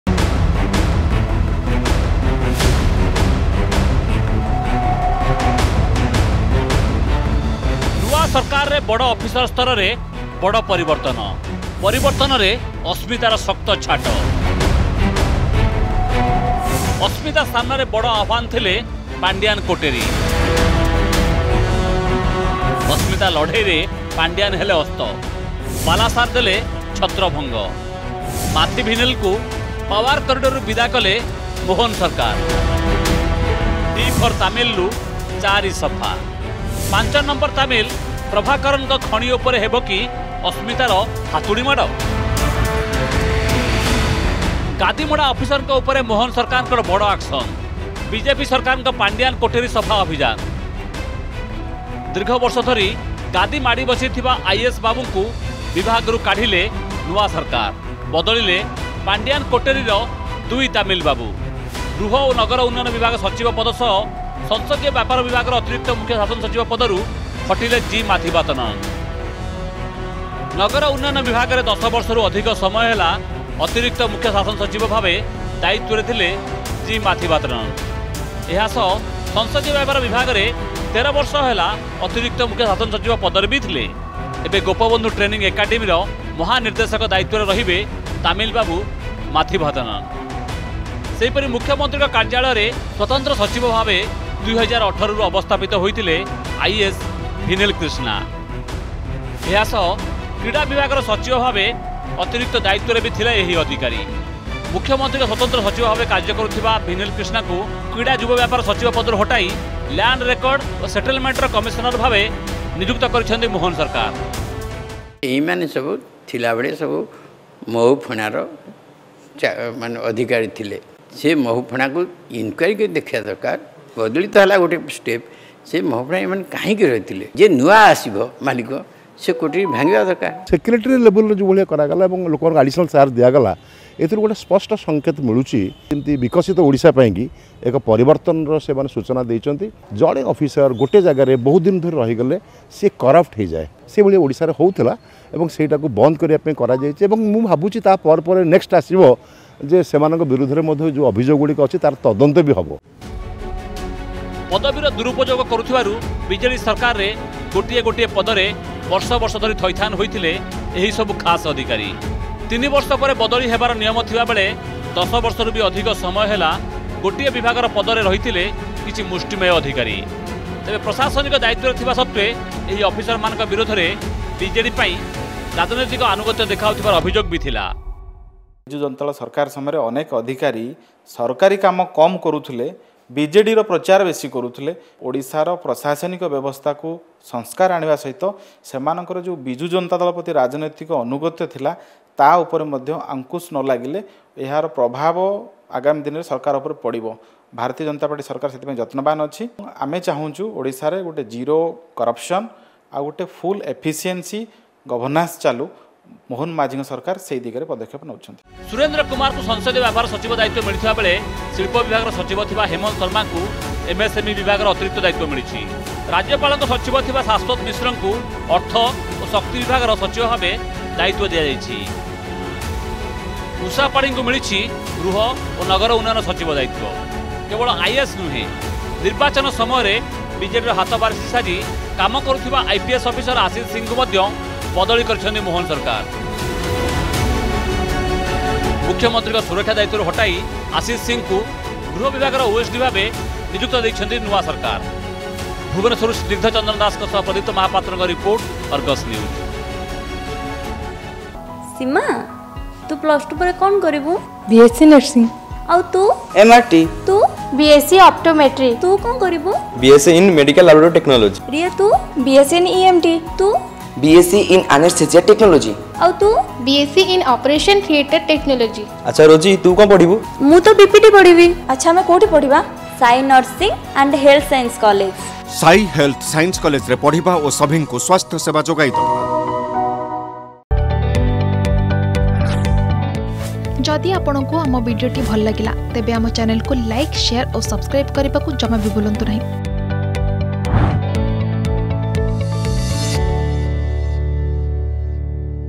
ନୂଆ ସରକାରରେ ବଡ ଅଫିସର ସ୍ତରରେ ବଡ ପରିବର୍ତ୍ତନ || Big Changes Senior Officers
ନୂଆ ସରକାରରେ ବଡ ଅଫିସର ସ୍ତରରେ ବଡ ପରିବର୍ତ୍ତନ ହୋଇଛି । ଯାହା ଅସ୍ମିତାର ଶକ୍ତ ଛାଟ । ଜି. ମାଥିଭାଥନନ ଓ ଭିନଲ୍‌କ୍ରିଷ୍ଣାଙ୍କୁ ପାୱାର କରିଡରରୁ ବିଦା କରିଛନ୍ତି ମୋହନ ସରକାର । T- ଫର୍‌ ତାମିଲରୁ ୪ ଜଣ ସଫା ହୋଇଛନ୍ତି ।